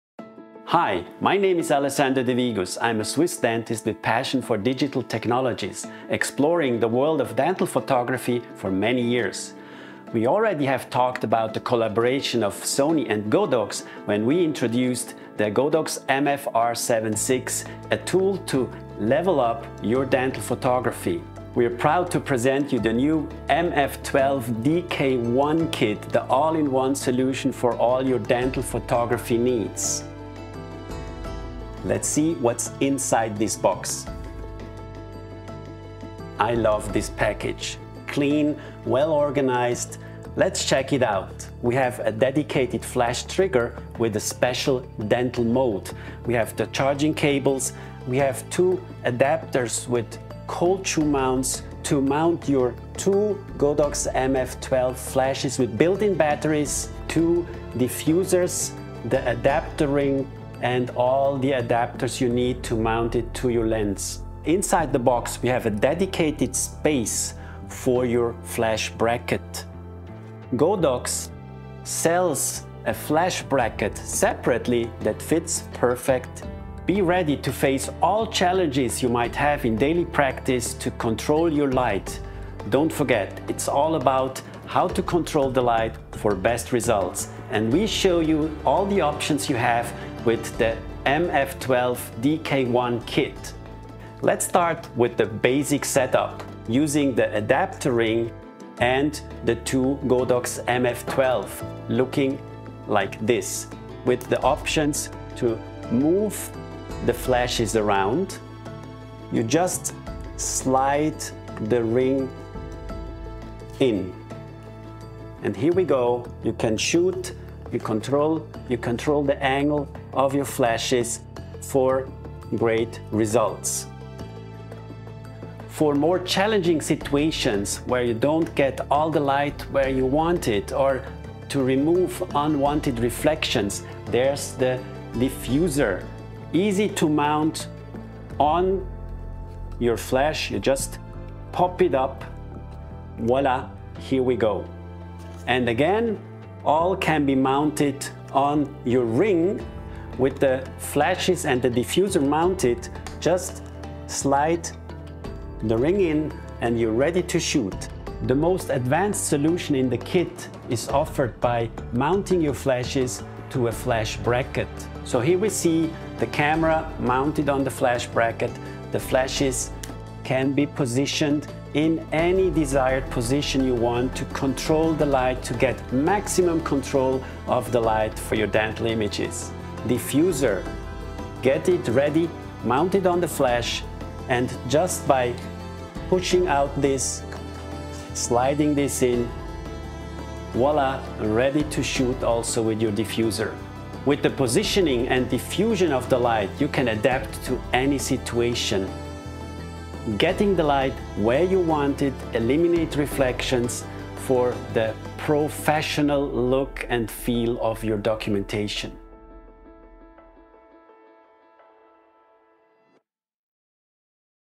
Godox MF12-DK1 System
Through extensive research and continuous refinement, in collaboration with Sony, we proudly
introduce the MF12-DK1 System, one more all-inclusive and advanced solution not only for esthetic dental photography. This comprehensive dental flash system includes all the accessories needed for dental photography, offering an all-in-one solution to address every possible lighting challenge in your daily workflow.